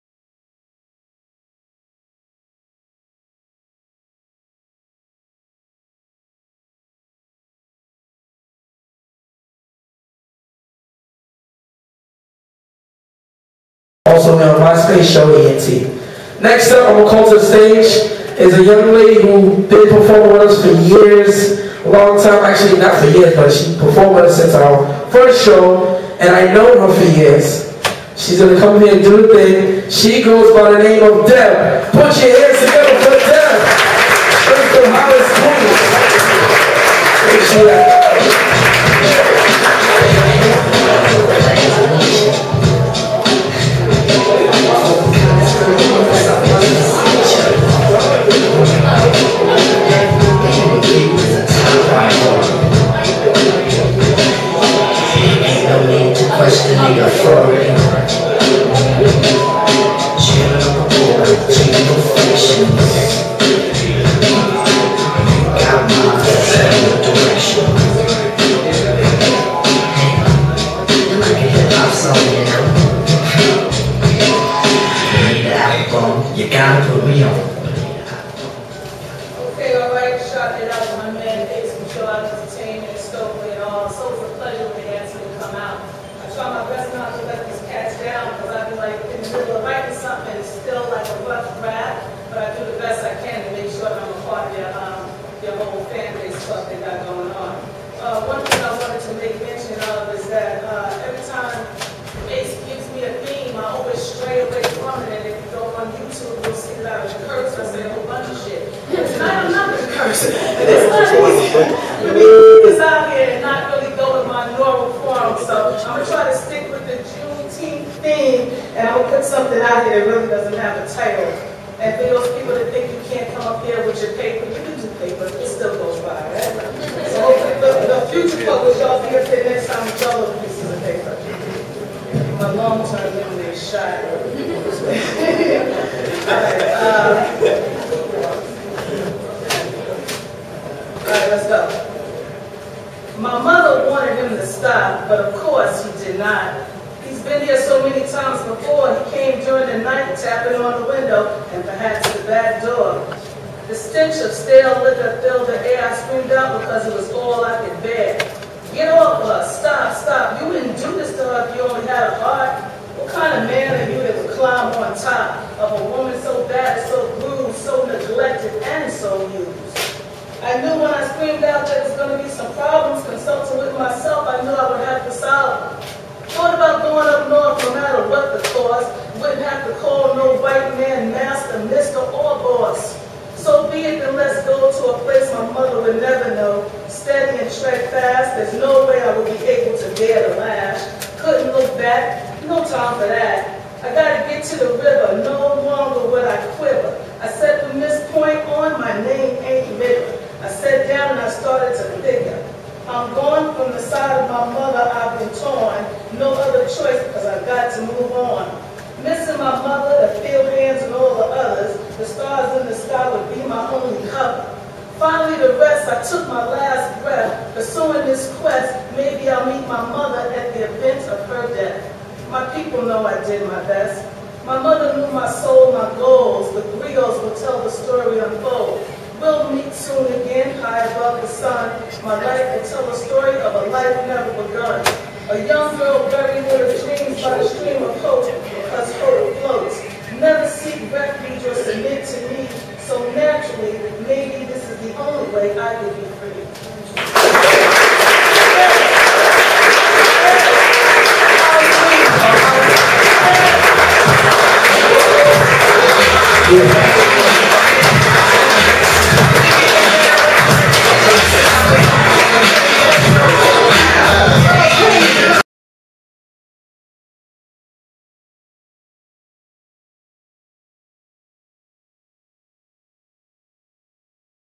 Deb performs at the Chashama Theater. At Show Off Ent's Juneteenth Celebration 6/19/2009
Show Off Entertainment Presents:
Juneteenth Spoken Word & Poetry Celebration

The Chashama Theater
217 East 42nd Street

Showent.com * Myspace.com/Showent
Youtube.com/Showent * Twitter.com/Showent

Performances by:
King Tut * P.O.E.T. * Mr. King * Mark Ross
Deb * Distinguish * Kapotheosis * Mrs. Busy Body
Rain * Ghost * Frank * Pins